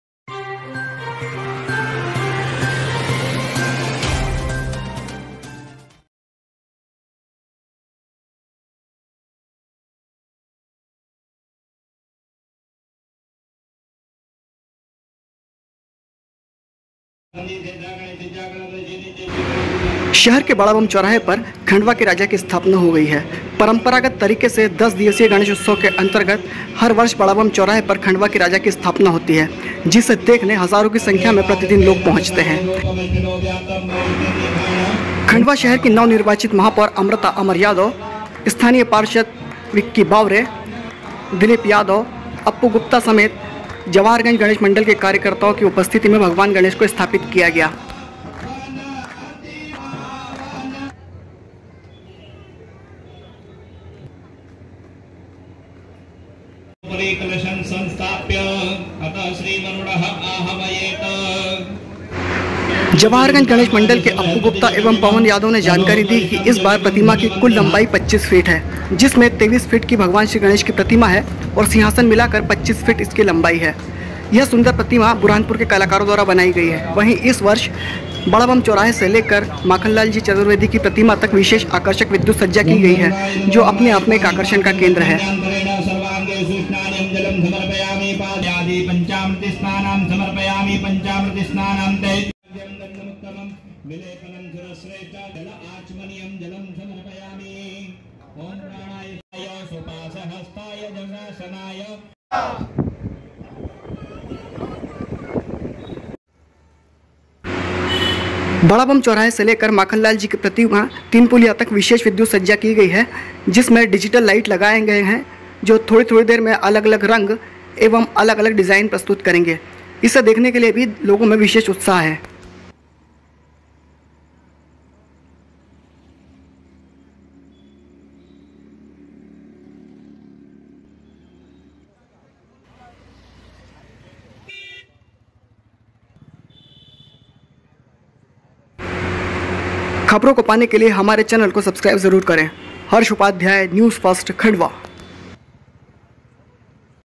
Khandwa-विराजे खंडवा के राजा महापौर रही उपस्थित, प्रतिमा और विद्युतसज्जा ने मोहा मन..
Khandwa-स्थापित हुए खंडवा के राजा महापौर रही उपस्थित, प्रतिमा और विद्युतसज्जा ने मोहा मन।@NewsfirstKnw